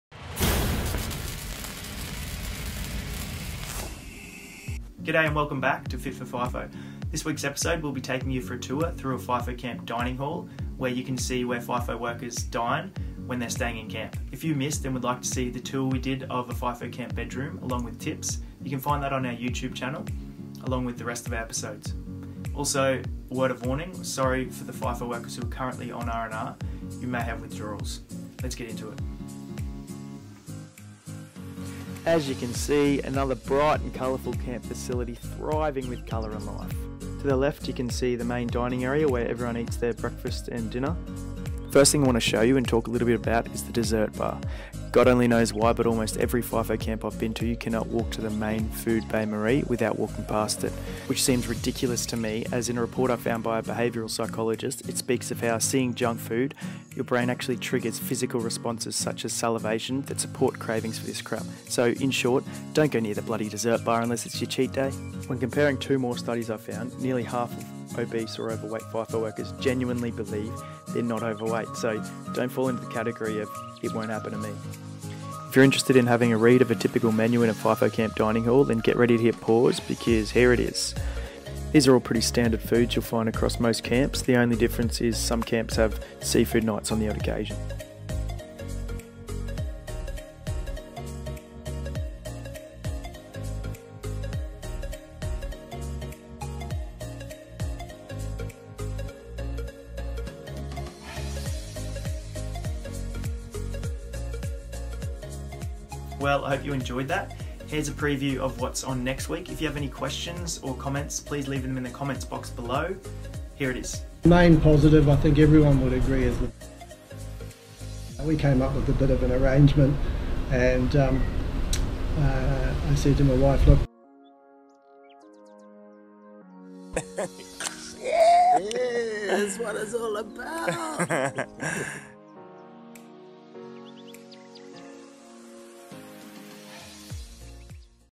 Ep.#6 FIFO Camp Food & Dining Tour & Tips
What does a Fifo camp dining hall look like? And what are some common traps? You are about to find out. Join Experienced Fifo worker Jason as he walks you through a Fifo camp dining hall and discusses a common trap for people in the FIFO industry.

Fit4FIFO is about life in the fly-in fly-out for mining industry professionals in Australia.